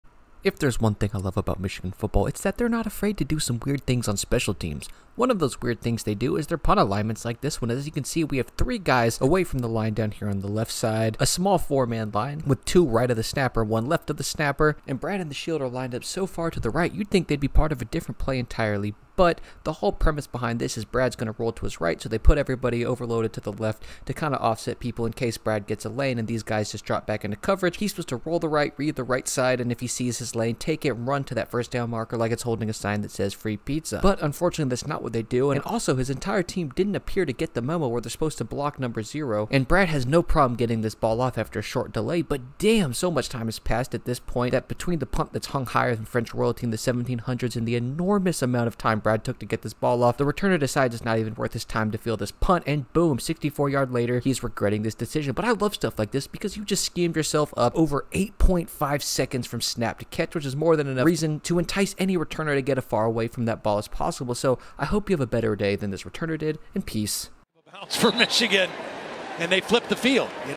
It Looks Ridiculous but is 1000 IQ!
Brad Robbins have yourself a day my man! Heck of a punt to follow up with this super weird punt scheme!